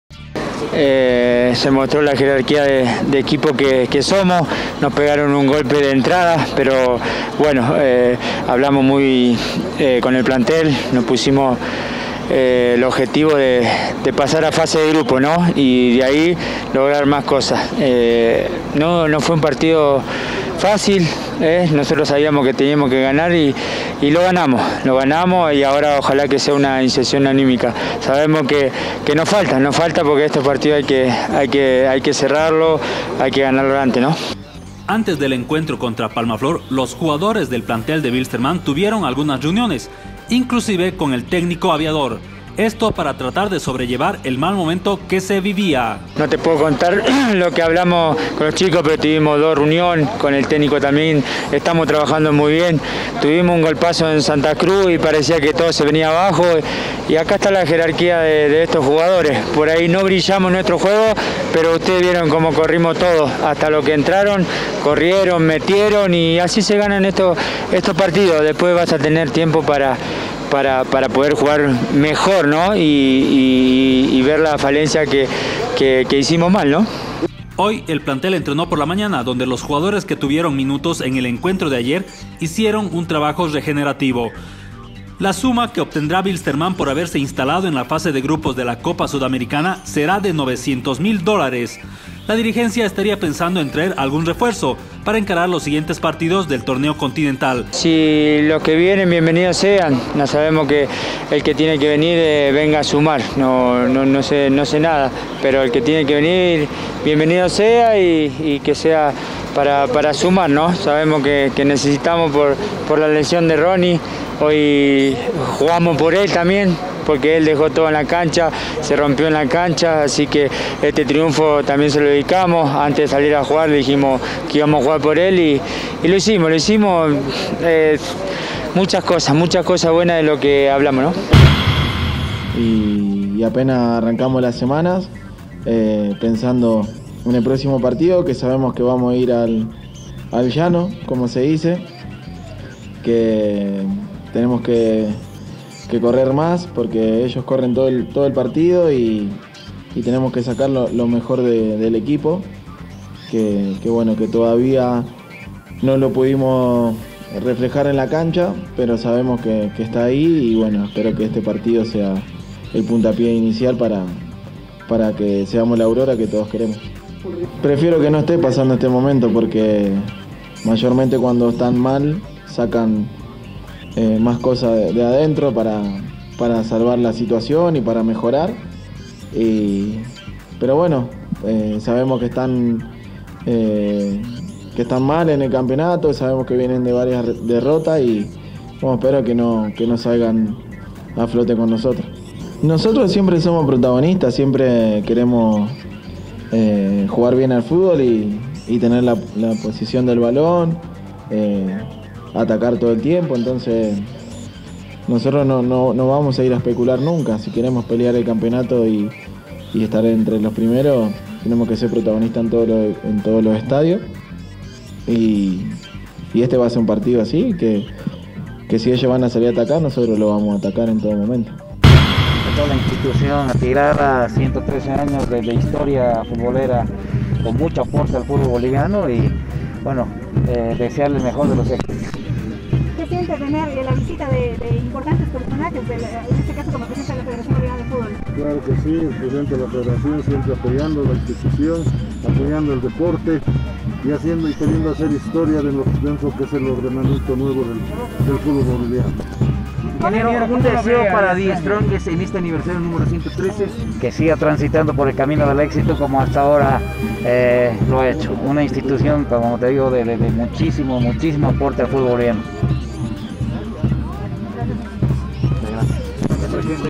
WILSTERMANN CLASIFICA - AURORA ENFRENTA A BLOOMING - THE STRONGEST COMPLE 113 AÑOS
para Cristian Chavez ganaron por la jerarquia desus jugadores - el plantel de aurora ya tiene todo listo para jugar contra blooming este viernes - el club The Strongest comle 113 años de vida institucional.